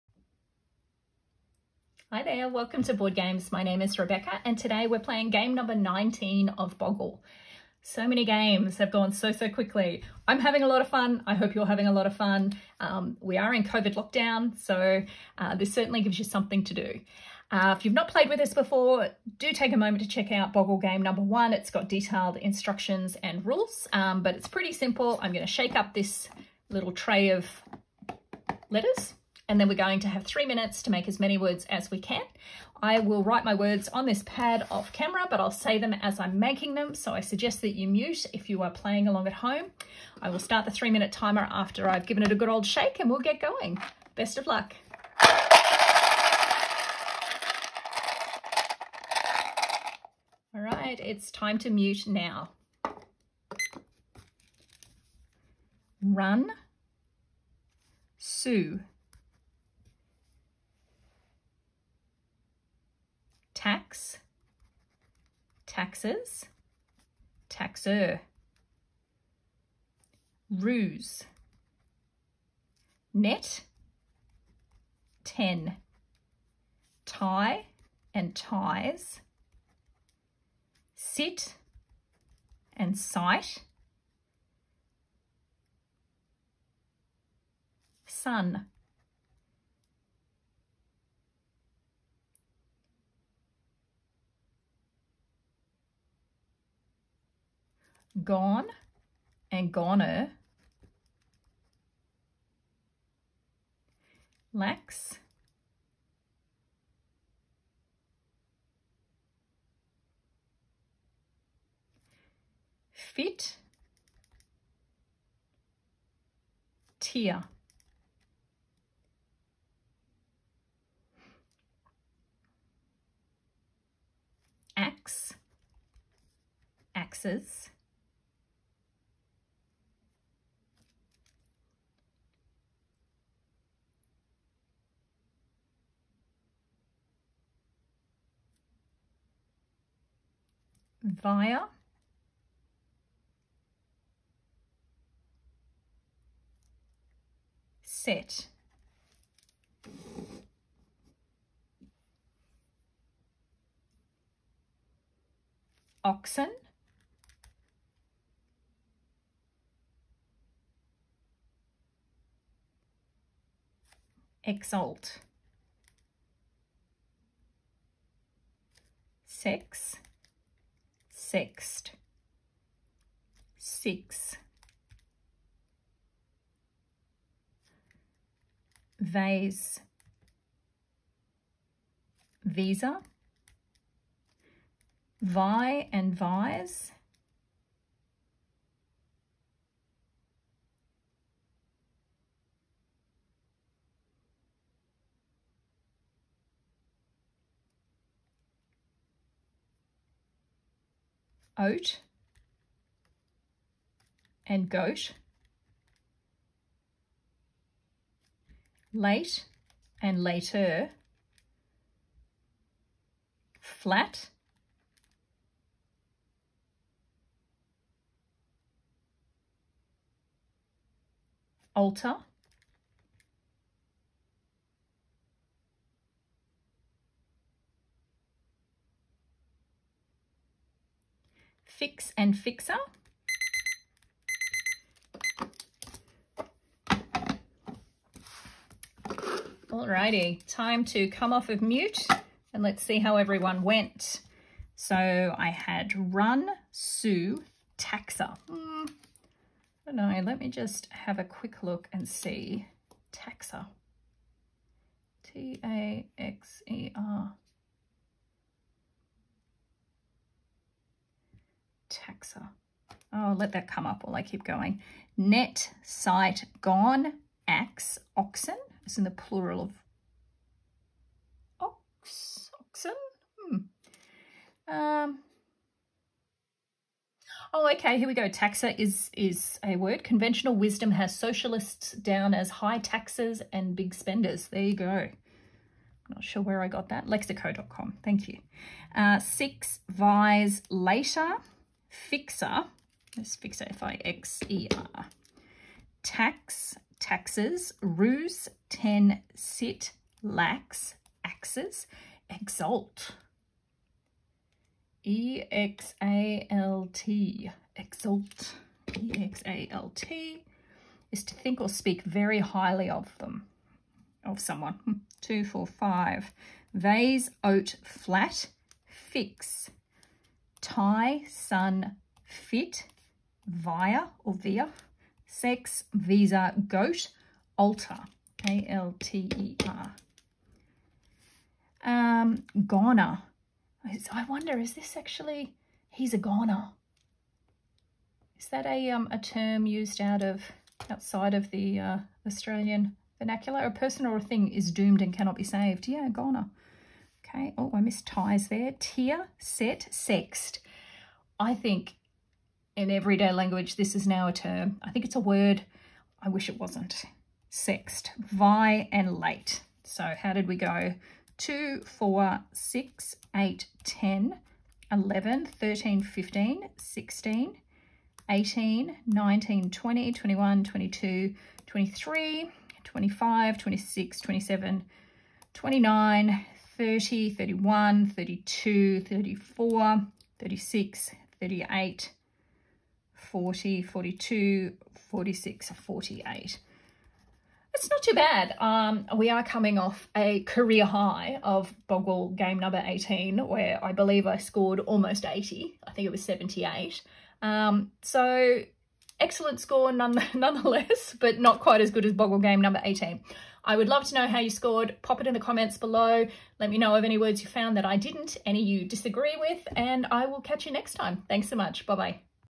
Boggle Game #19
Join me for a game of Boggle... wherever you are in the world!

My score for this game was 48. I would love to know how you scored. Please share in the comments below.

Standard Boggle like we played today
Big Boggle
Super Big Boggle
Boggle Junior